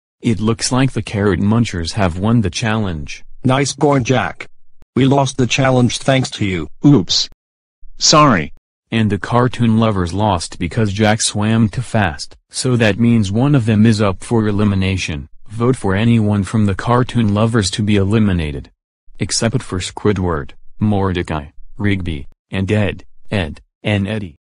character elimination ep 8: part 5
time to vote for one of the members from the cartoon lovers to be eliminated